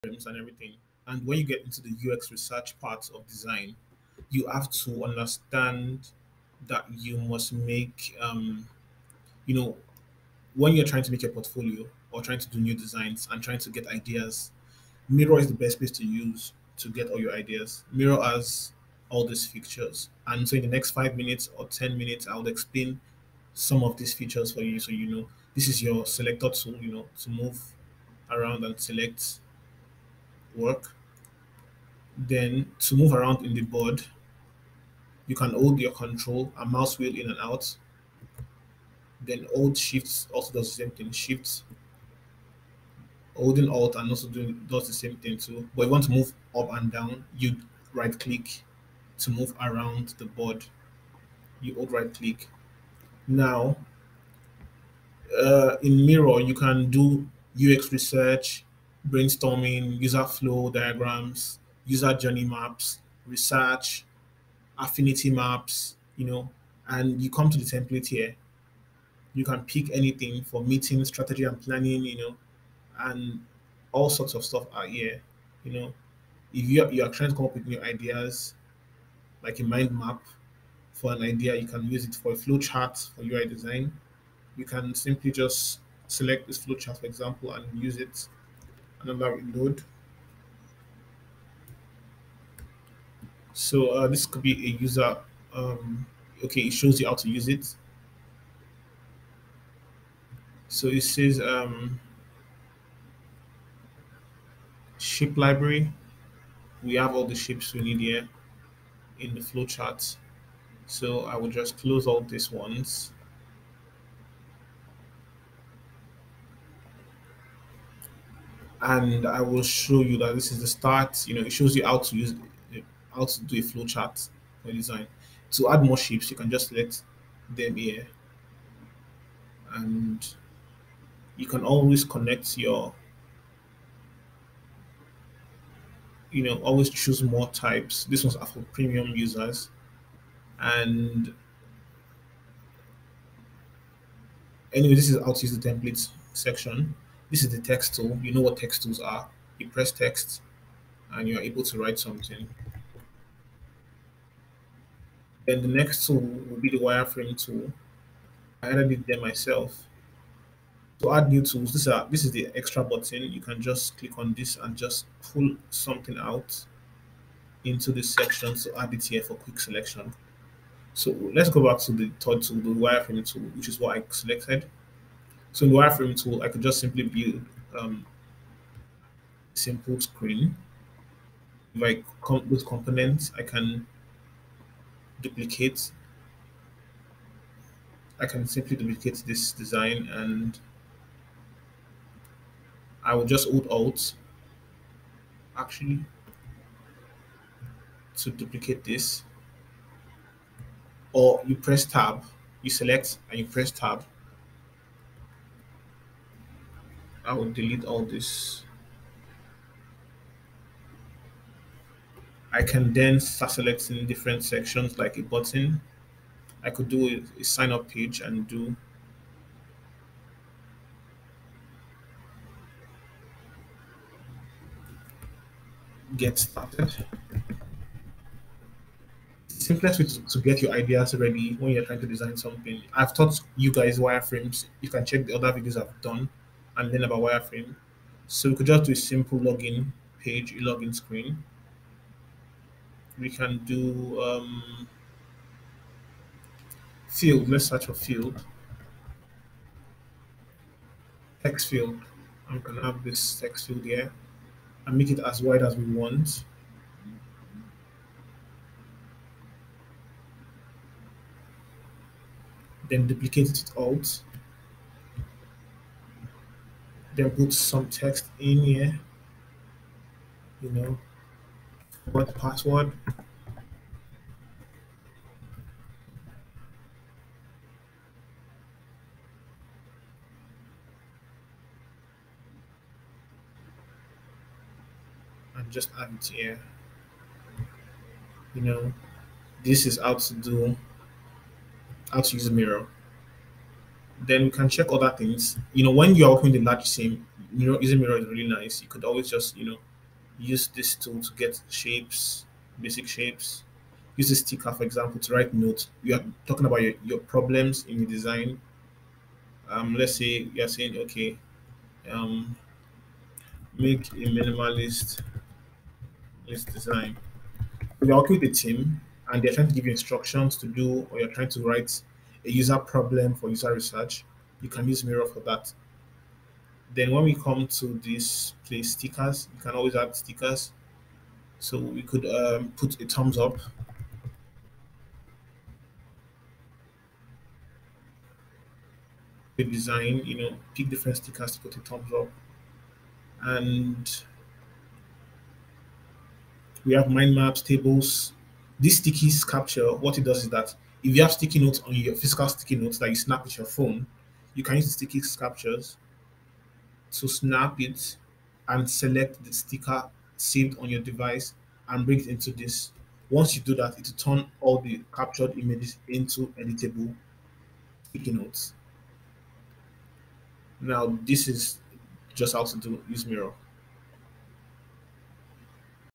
How to use Miro for wireframes
Here is a quick process on how to use Miro for UX research and to make Wireframes.